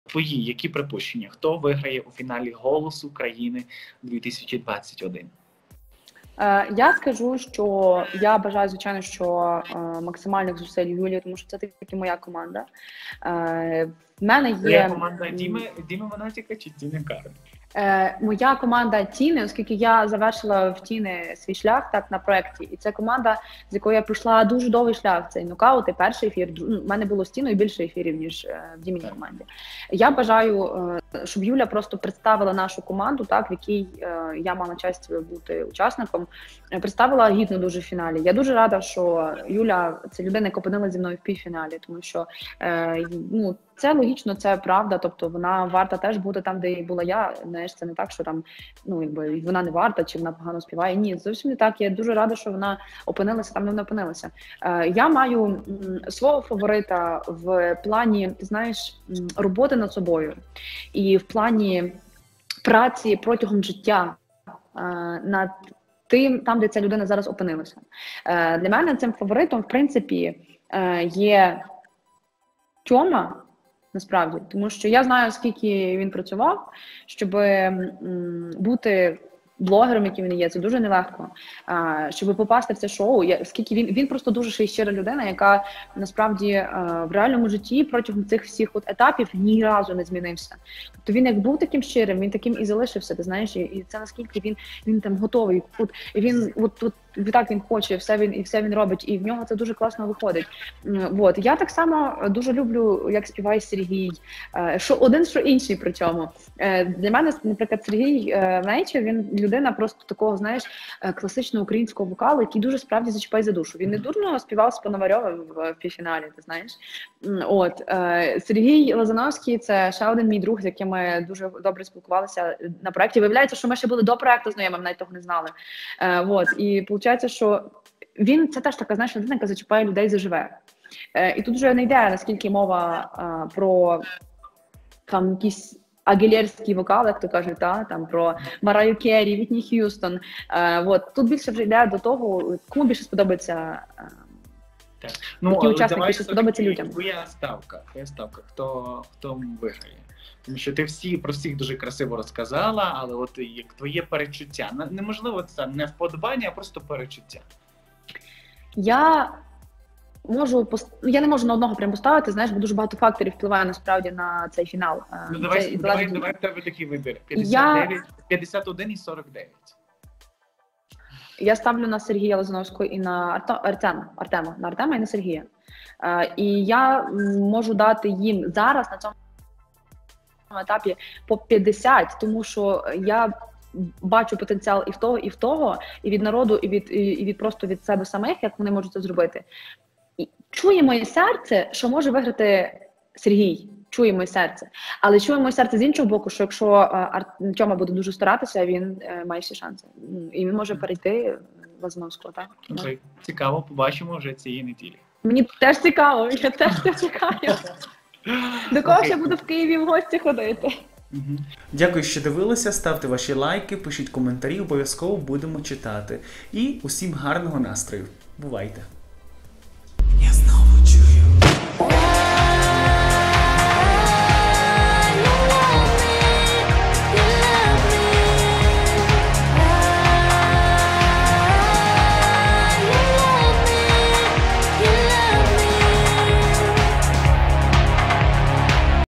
Хто переможе у фіналі "Голосу країни" 11? Півфіналістка Катя Гладій називає претендента на перемогу
Катерина Гладій аналізує 4 претендентів на перемогу в 11 сезоні "Голосу країни". Хто на її думку зможе перемогти: Юлія Тимочко, Сергій Нечаєв, Тьома Паучек, Сергій Лазановський?